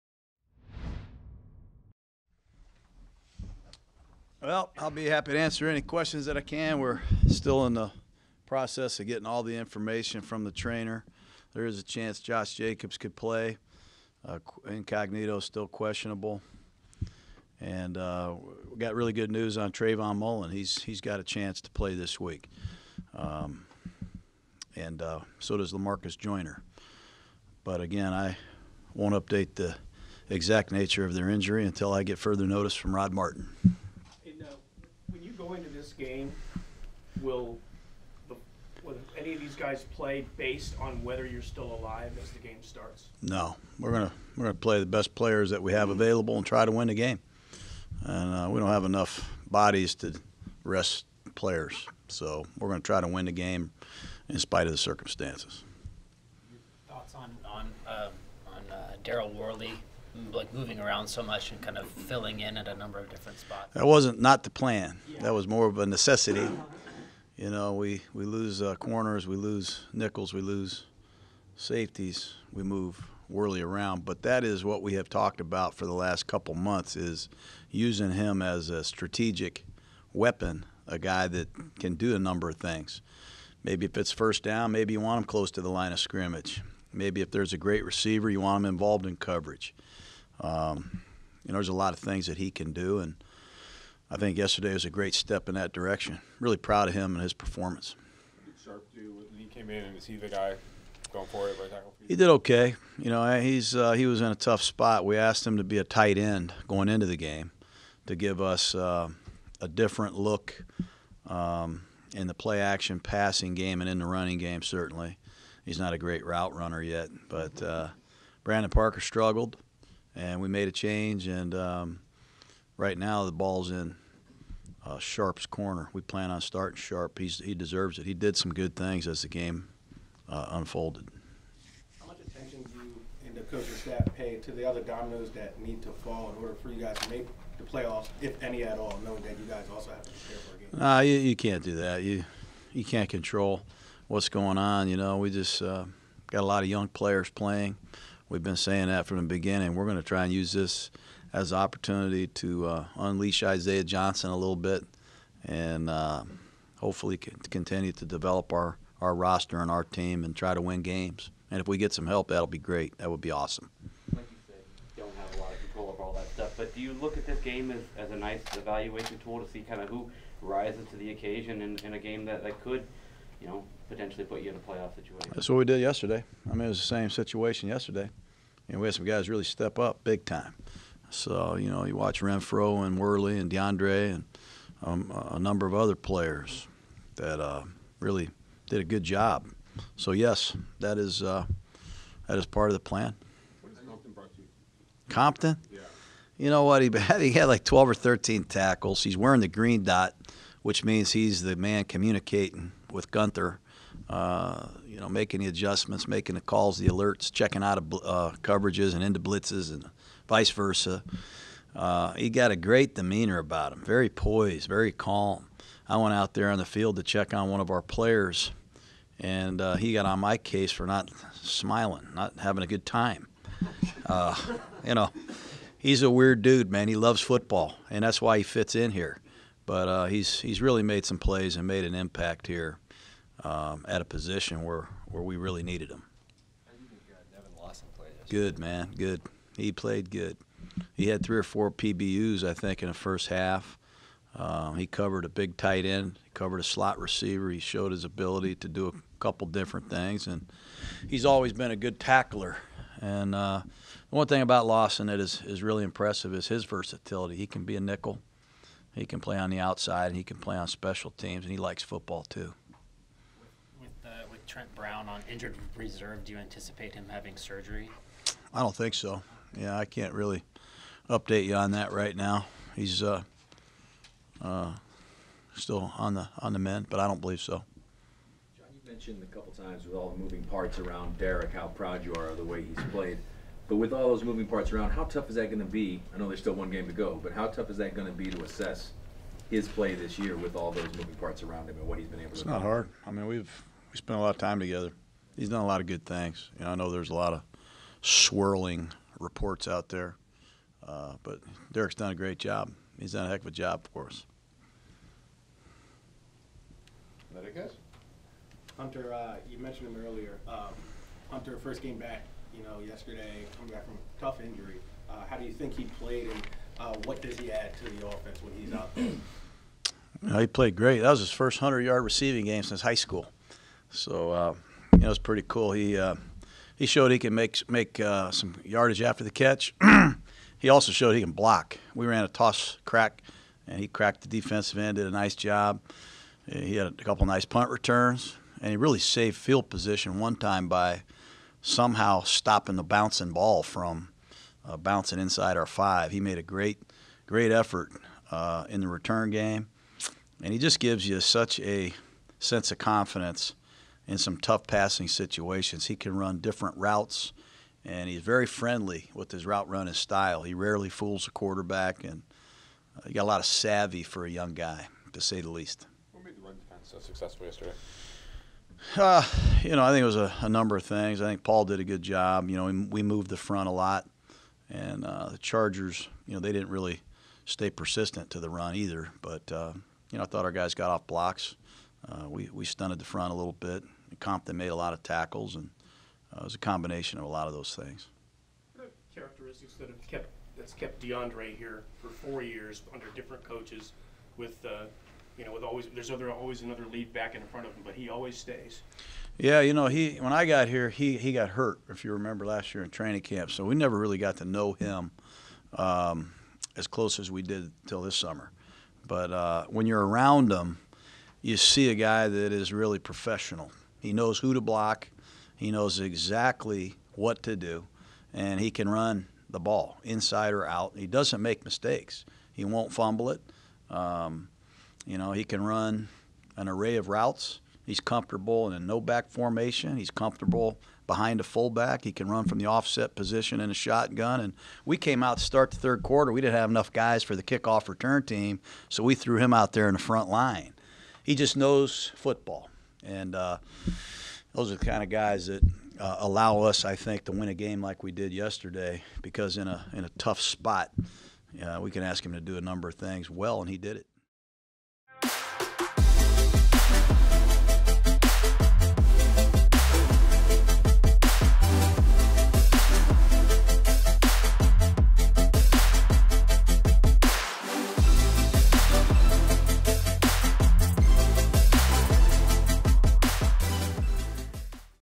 Coach Gruden Recaps Week 16 Win & Updates Josh Jacobs Injury | Raiders
Head Coach Jon Gruden discusses the Week 16 victory over the Chargers, wide receiver Hunter Renfrow, the versatility of cornerback Daryl Worley, and more.